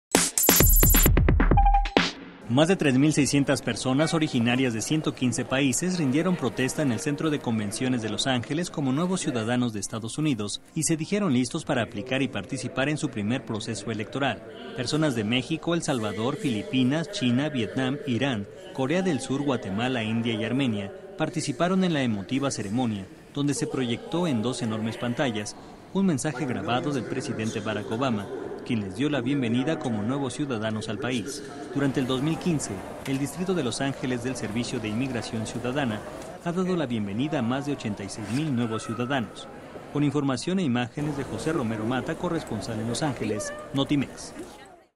3600 personas rindieron protesta como nuevos ciudadanos de Estados Unidos
En la emotiva ceremonia se proyectó un mensaje de bienvenida del presidente Obama